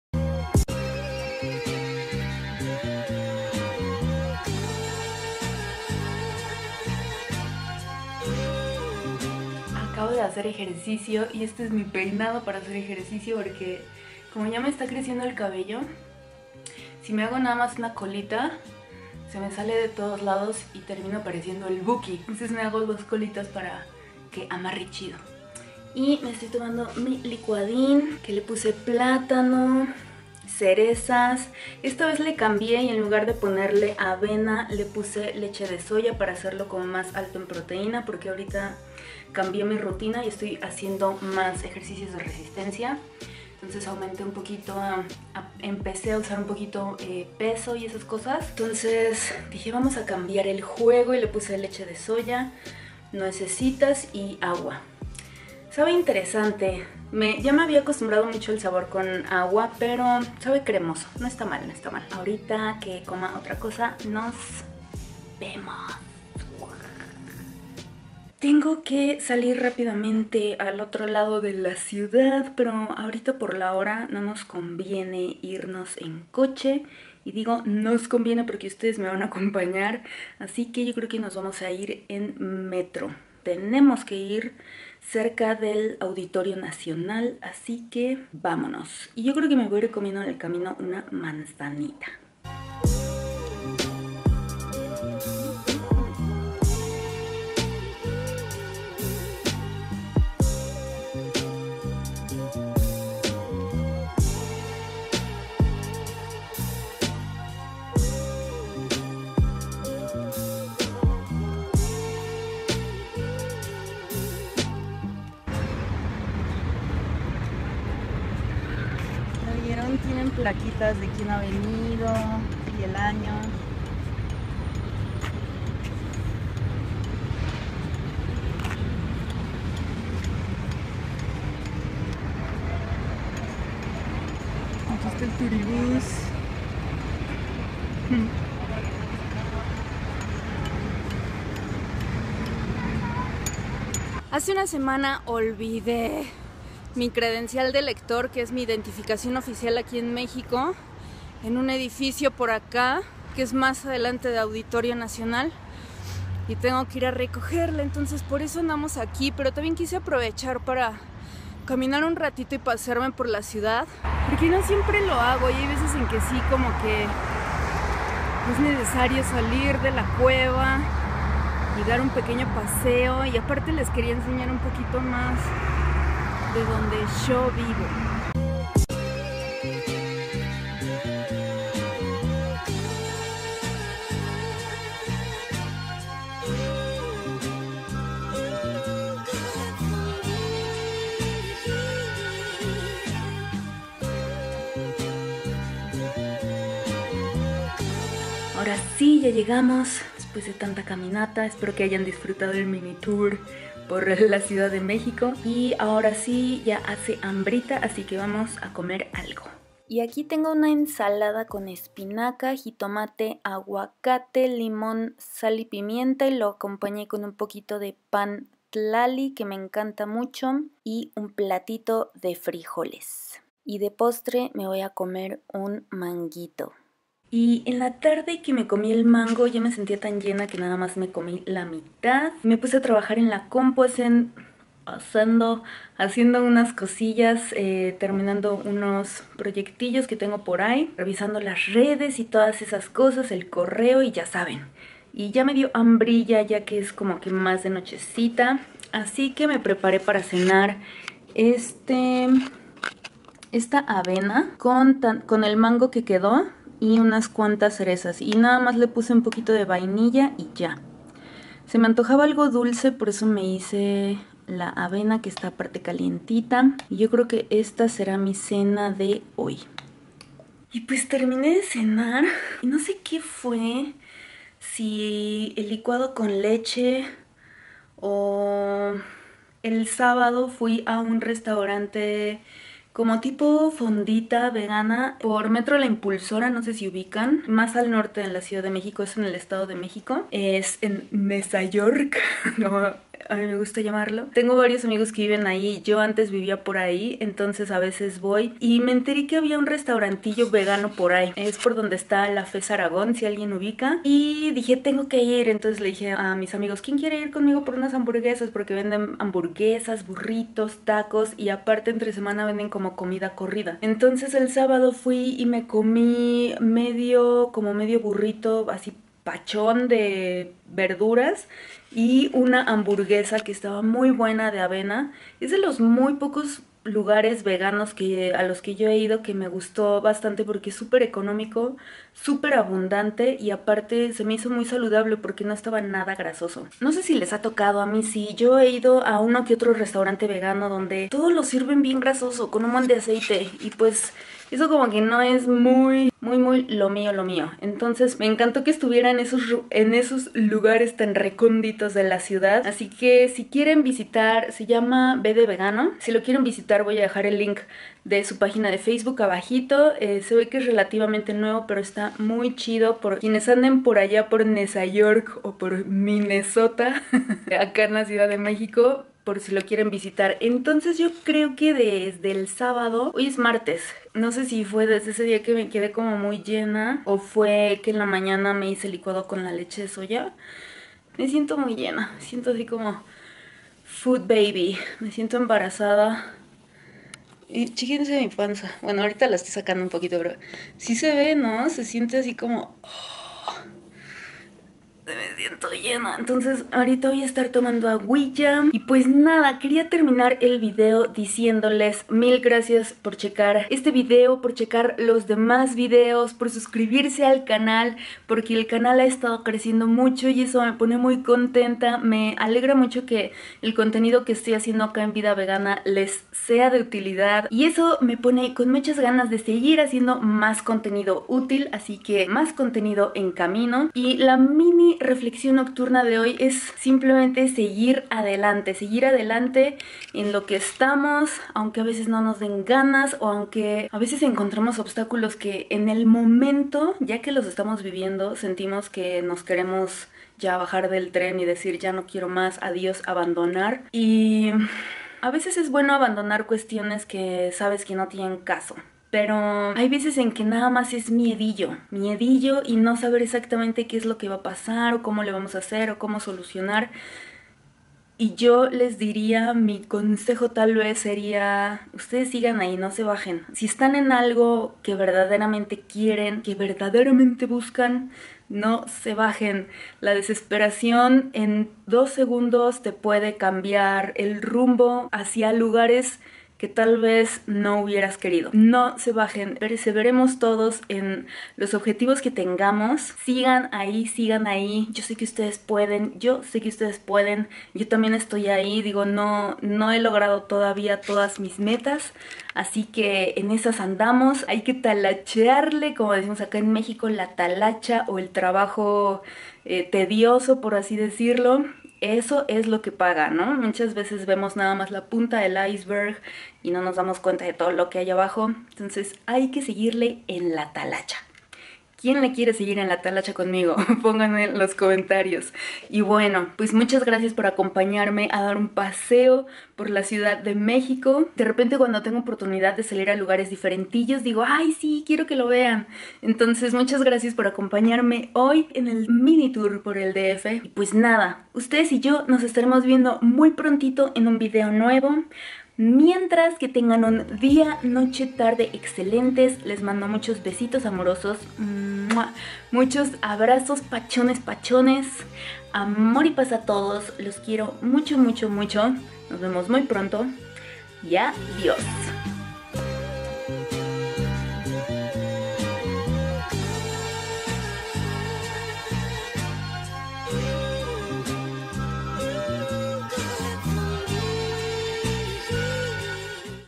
QUÉ COMO EN UN DÍA - VEGANO + TOUR POR LA CD. DE MÉXICO - VIDA VEGANA
MÁS RECETAS Y VIDEOS